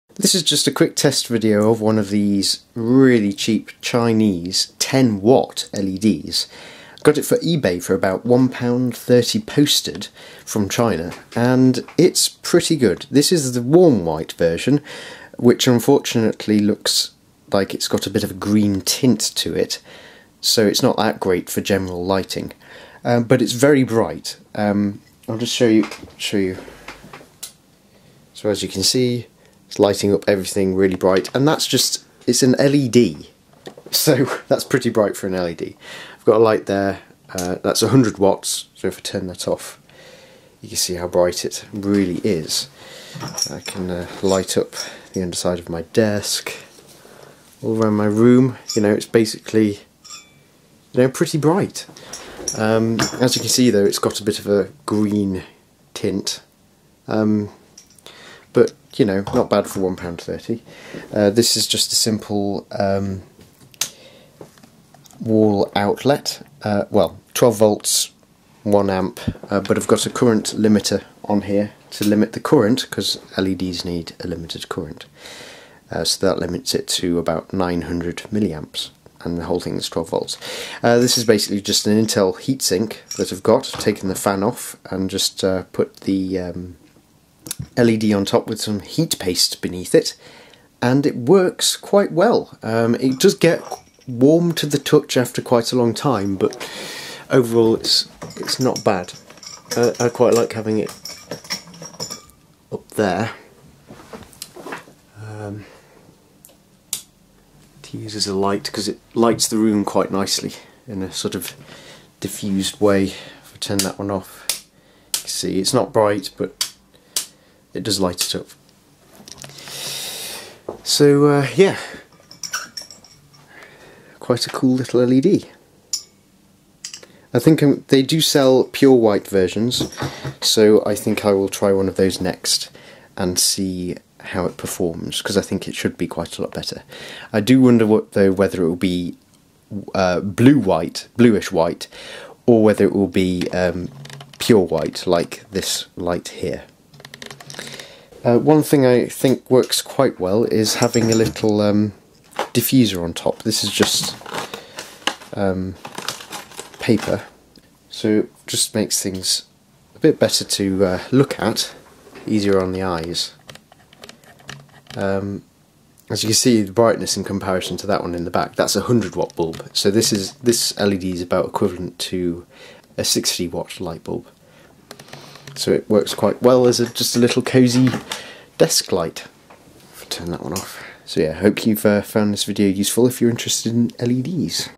10w LED (very bright!)
Testing out a 10w LED from China.